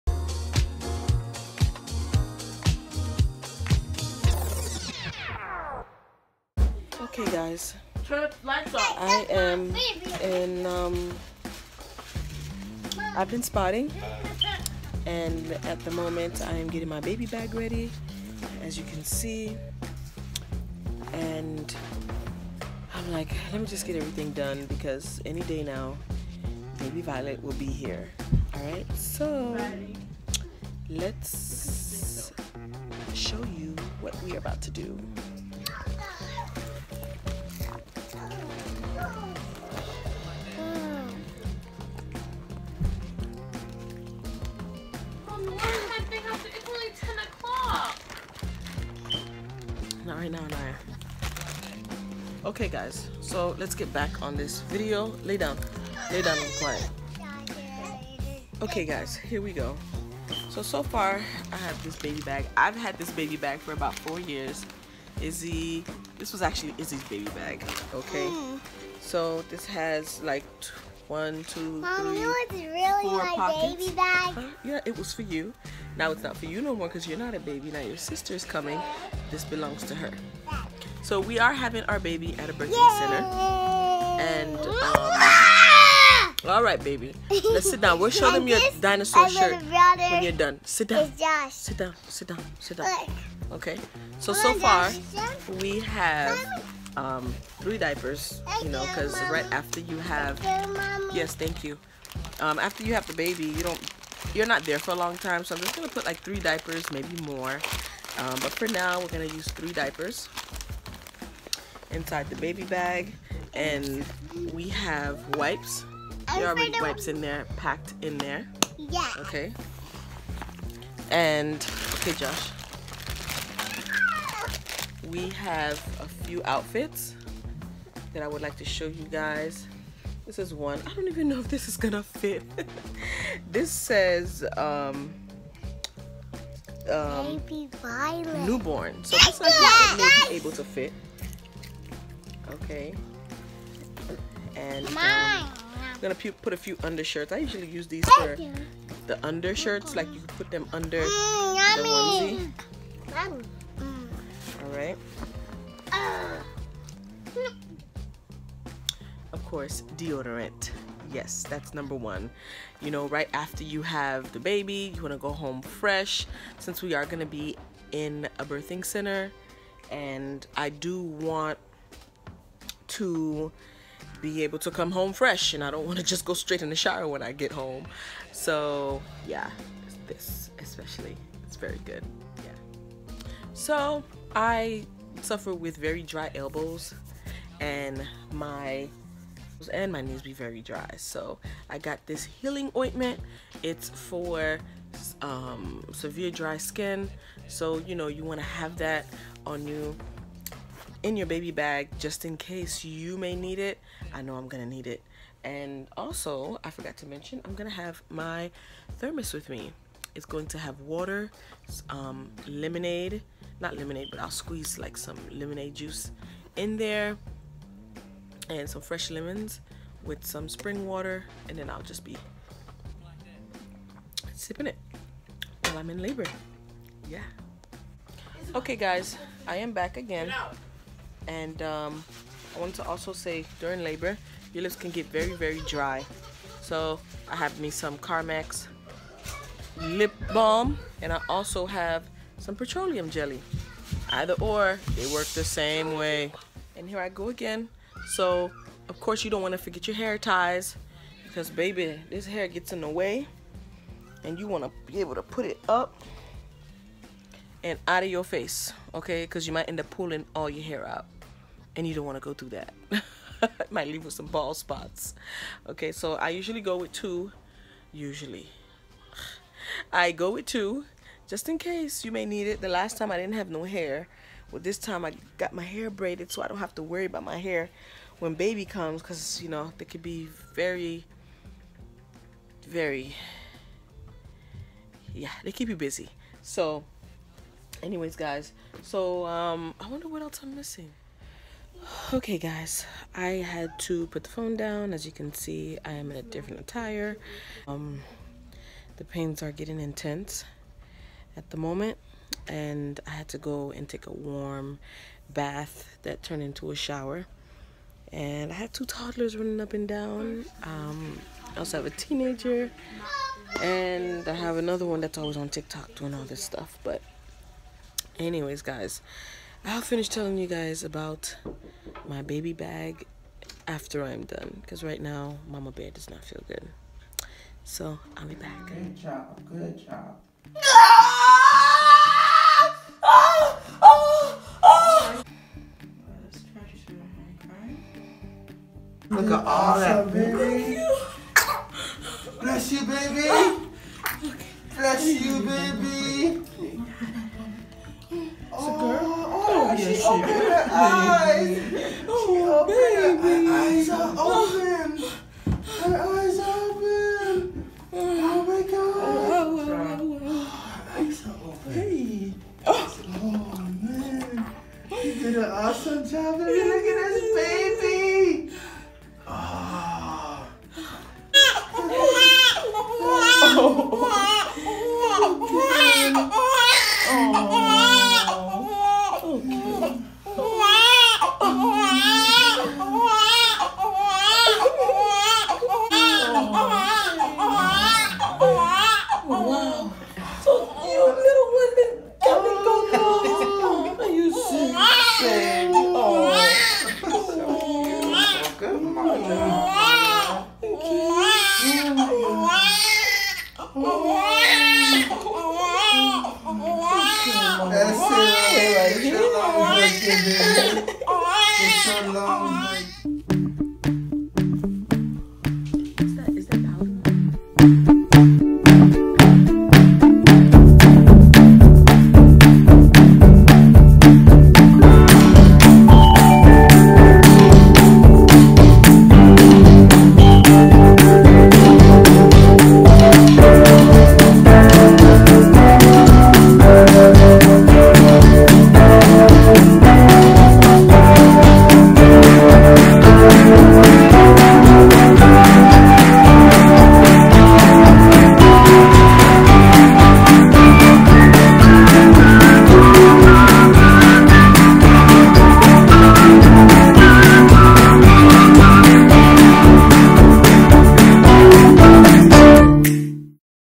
Packing my baby bag!
I was due to have Baby Violett August 22. Instead she decided to come August 8th at 3:01AM. I was 38 weeks pregnant. We are so overjoyed to have had a successful water birth with our midwife and her assistant. Thank you guys for watching.